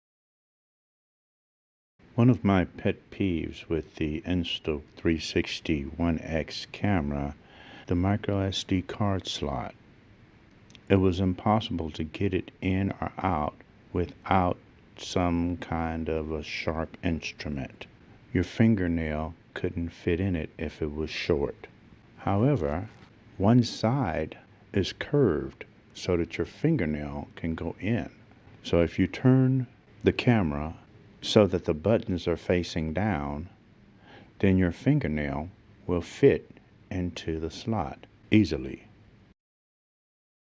Insta360 One X SD card slot
This is my first YouTube video. Gonna start off short and sweet.
I've had the Insta360 One X camera for 16 days. I love it. Buuuuuuuut, the micro SD card slot is too small, or so I thought, until I looked at it closely.